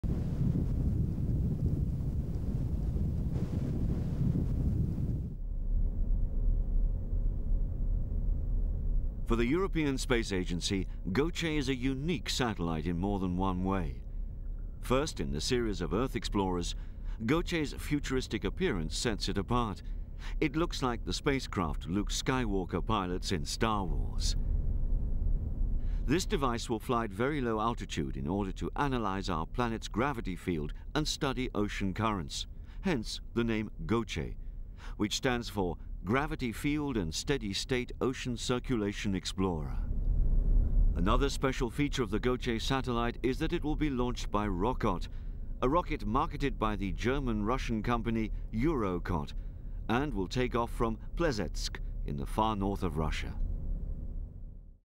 Introducing the GOCE Earth Explorer satellite
To achieve its crucial scientific objectives, ESA's 'Gravity field and steady-state Ocean Circulation Explorer', or GOCE, satellite must orbit as low as possible, in order to sense minute variations in the Earth's gravitational field - at the edge of space and the limits of the atmosphere at only 268 km!